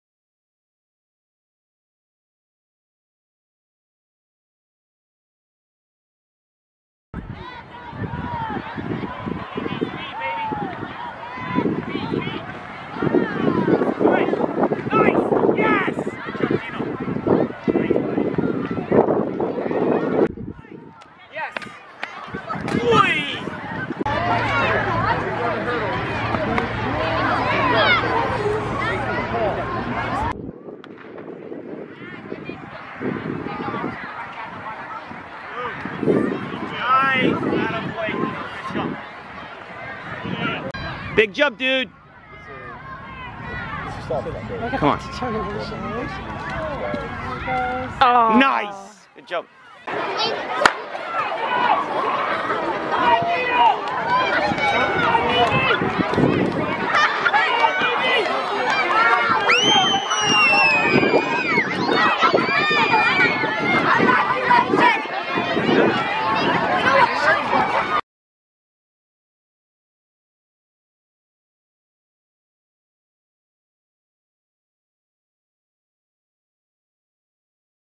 Dino Bontis - Track & Field Highlights (CISSA 2015) - Instagram: dinobontis
This compilation video shows highlights of Dino Bontis (Instagram: dinobontis) competing at the CISSA Track & Field Championships on May 20, 2015.  Dino competed in the U11 boys division.  He attends Hillfield Strathallan College.

RESULTS
High Jump - 3rd (tie)
100m - 3rd
Triple Jump - 2nd
Long Jump - 1st (new CISSA Record)

Dino is currently the U10 boys and U11 boys CISSA record holder in the long jump.

Dino plays goalkeeper for the Toronto FC Academy.